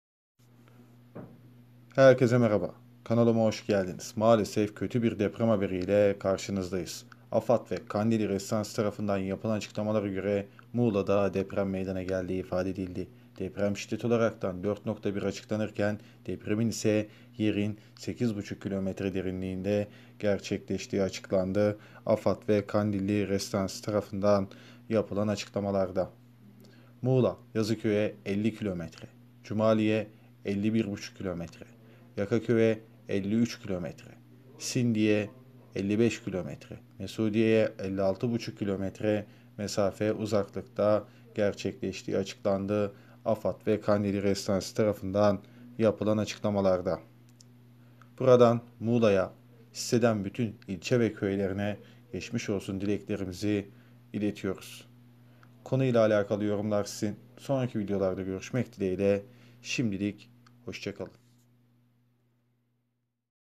Korkutan DEPREM! O İlimizden ŞİDDETLİ Korkutan DEPREM! Son Dakika Haberi
AFAD ve Kandilli Rasathanesi Depremi şiddeti kaç?
2024 son depremler şu şekilde. Depremi şiddetleri 4.2 olarak açıklandı.
Depremin 9 kilometre derinlikte olduğu açıklandı